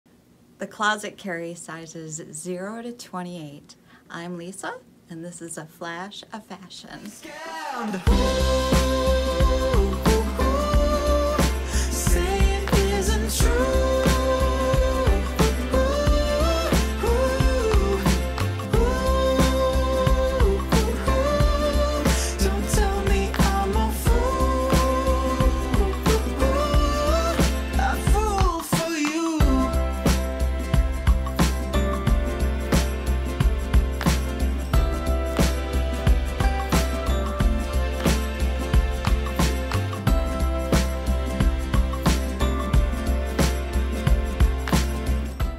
The Closet by A Beautiful Me / Flash of Fashion 109
The Closet by A Beautiful Me is located in Downtown Port Huron and has OVER 1,000 dresses on the floor! Pick a dress that matches your personality and make a statement.

Women of ALL ages can have fun with patterns and color! Find new ways to express yourself with the dress you choose!

Open weekdays 12-6 pm / Saturday 10-3 pm at 235 Huron Avenue, Port Huron, MI 48060